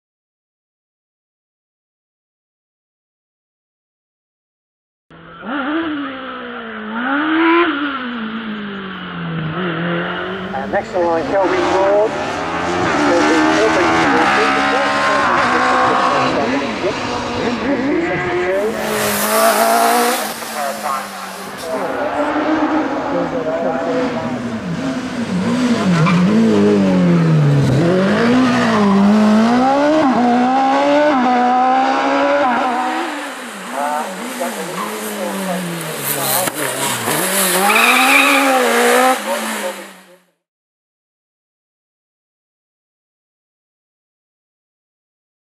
OMS CF04 At the National Championship Wiscombe Park 2014 Neal Coles
HD footage of Neal Coles driving his OMS CF04 At the National Championship Wiscombe Park 2014.